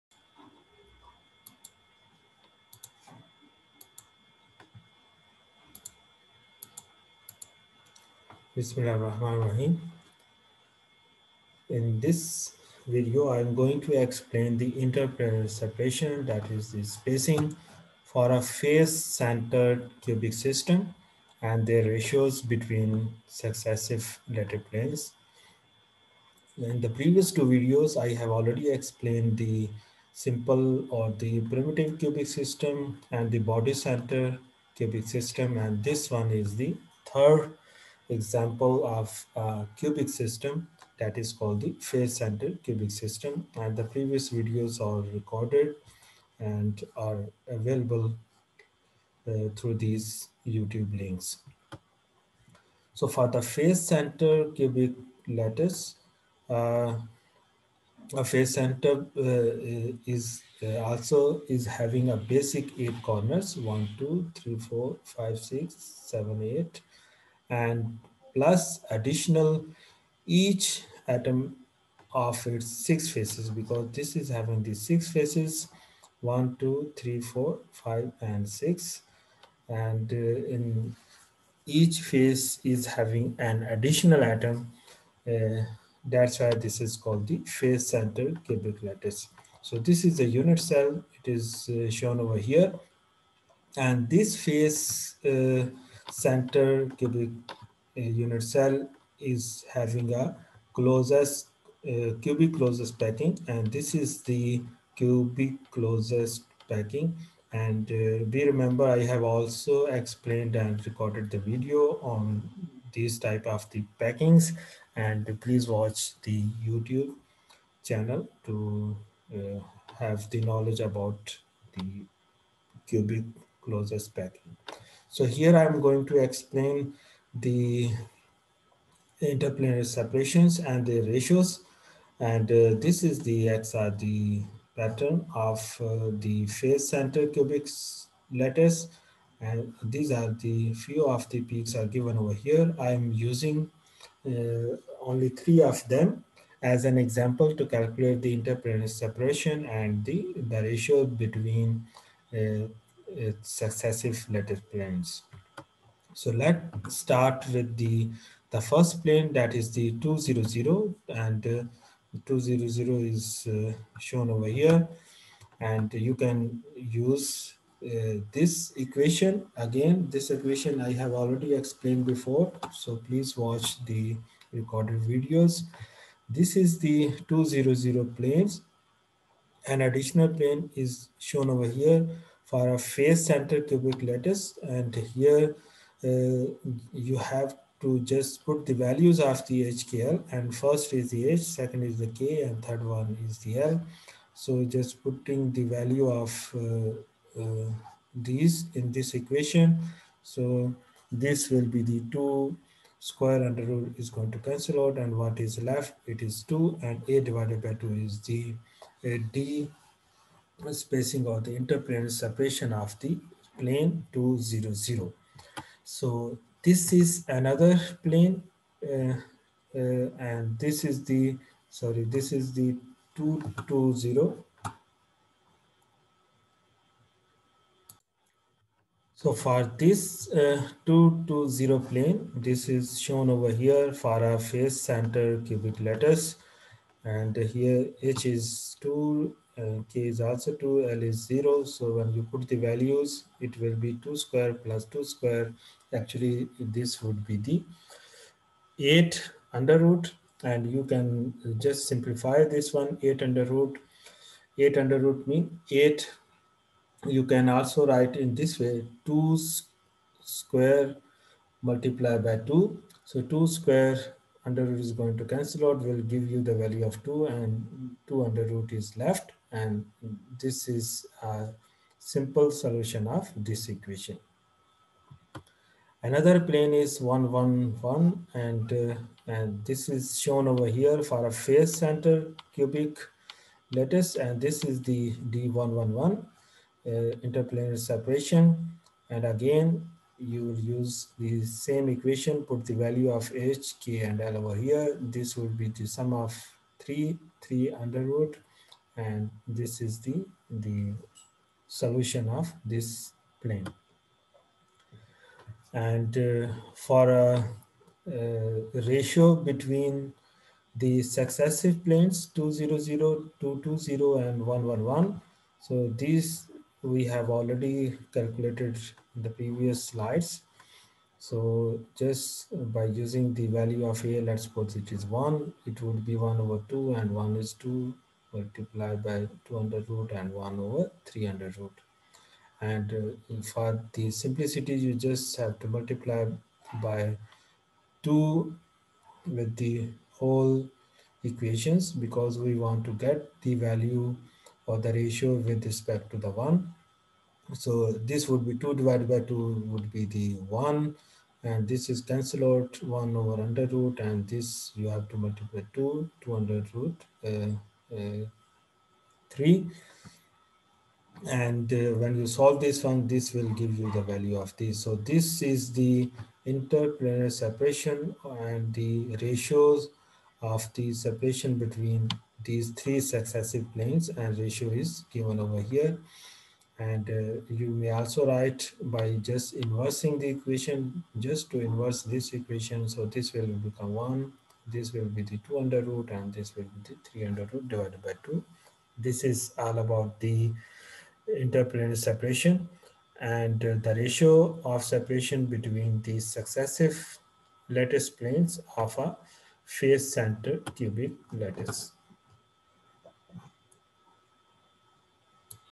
Interplanar separations and ratios of face-centered cubic system
Calculations of interplanar separations (spacing) for face-centered cubic system and their ratios between successive lattice planes